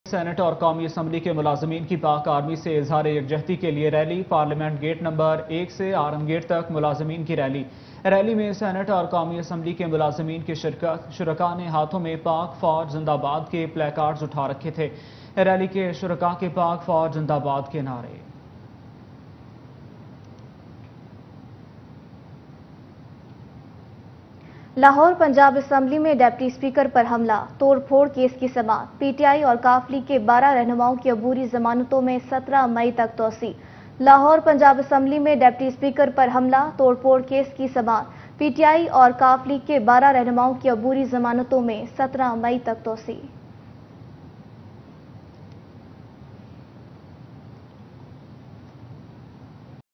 Senate and National Assembly employees rally to show solidarity with Pakistan Army
سینیٹ اور قومی اسمبلی کے ملازمین کی پاک فوج سے اظہار یکجہتی کے لیے ریلی